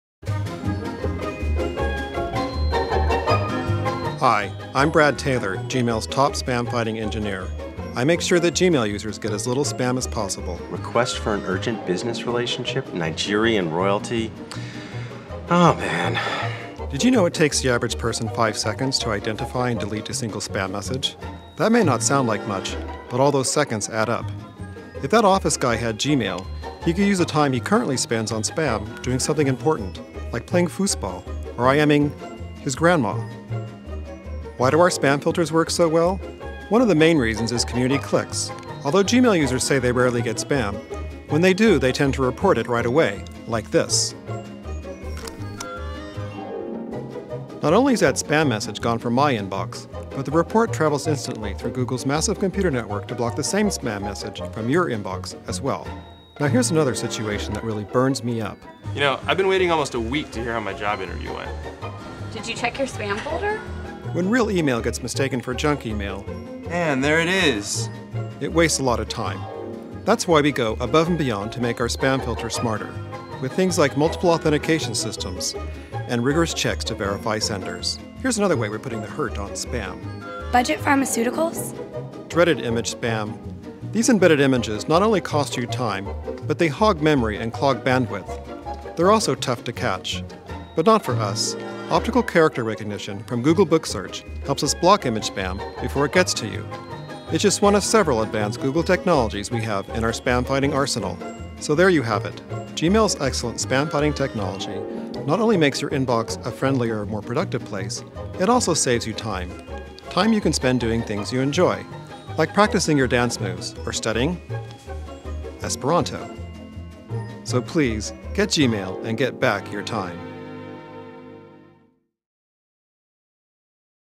Spam, time, and you: An educational video from Gmail
Learn how Gmail's spam-fighting technology saves you time.